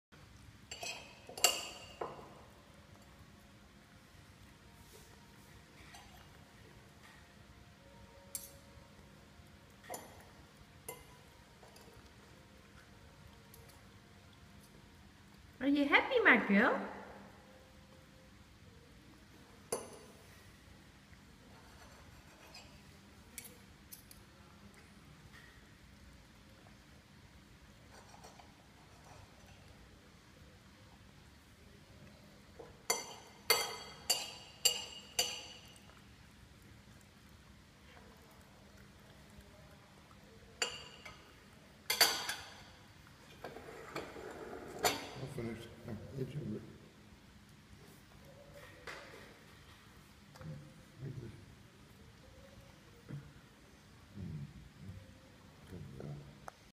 Border Collie eating breakfast at the table
My border collie puppy YOKO, eating breakfast with her daddy at the table - A developing morning ritual